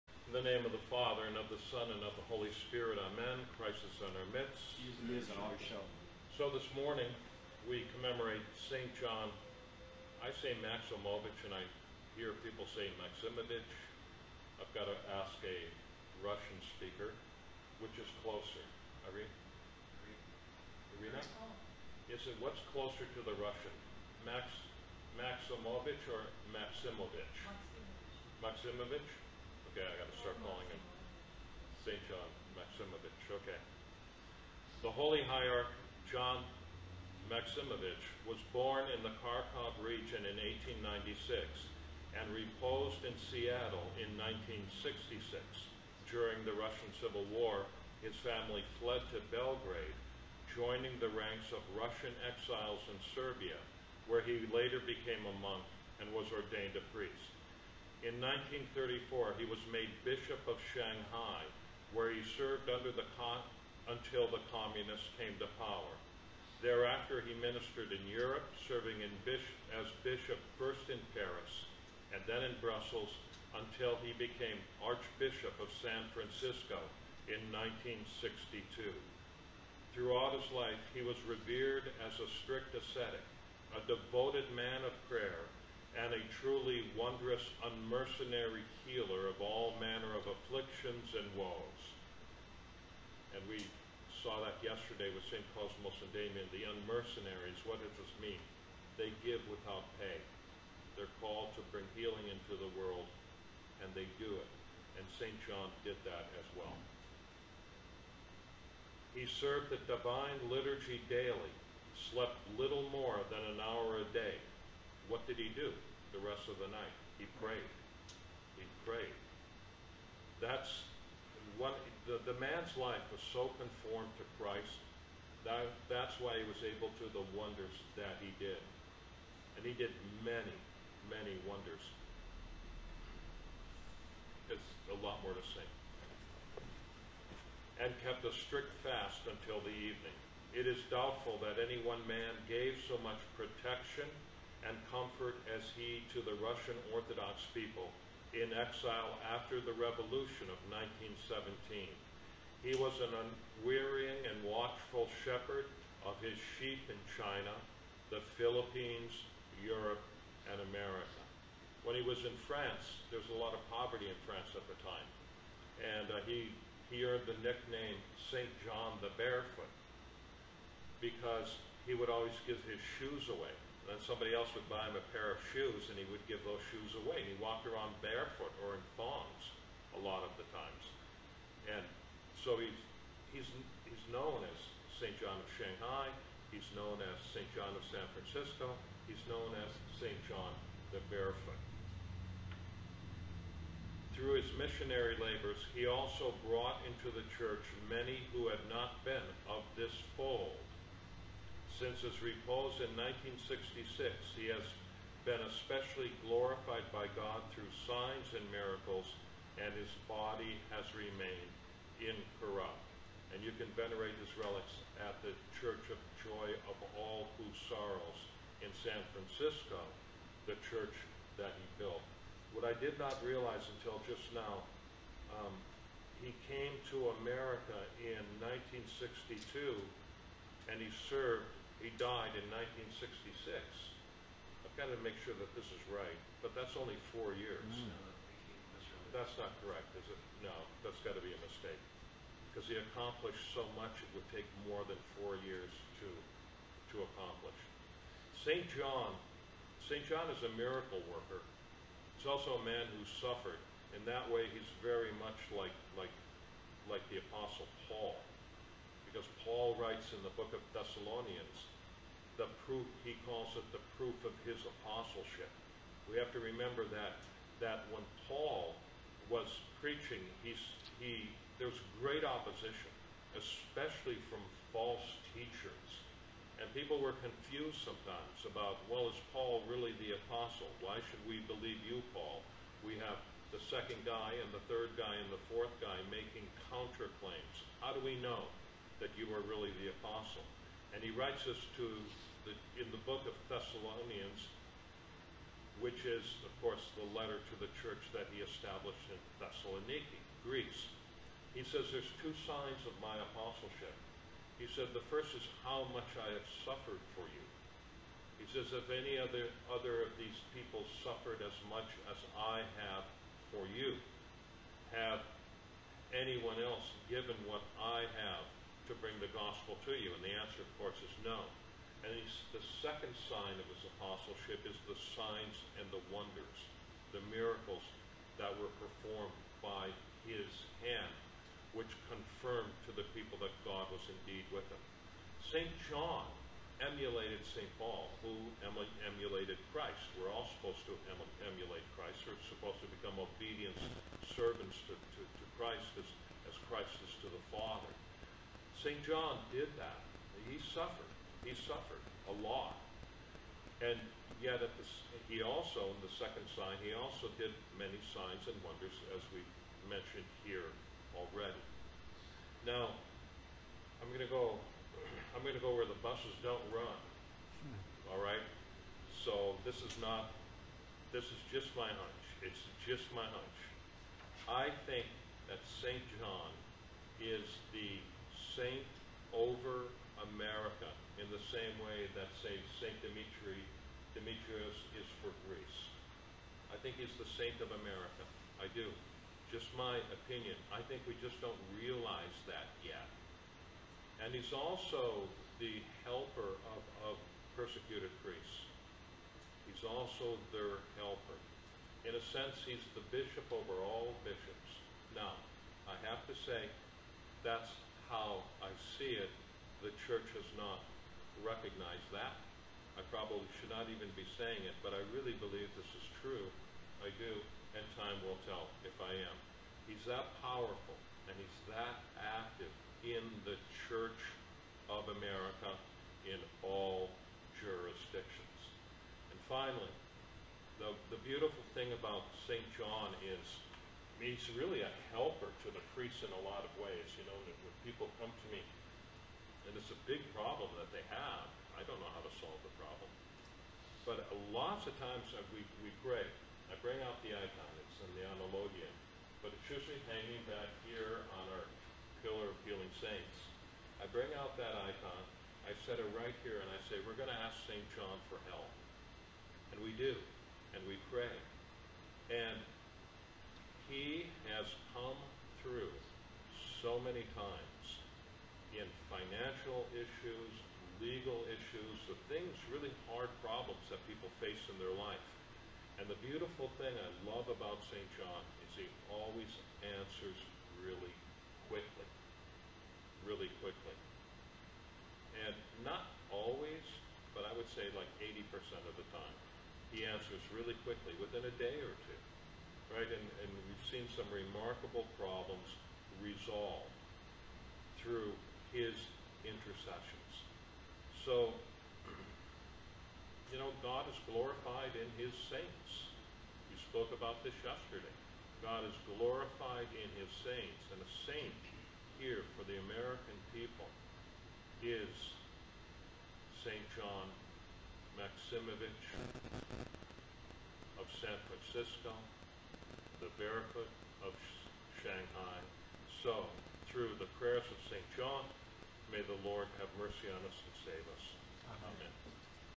The Saints Are Our Helpers
Fr. Hans talks about St. John Maximovitch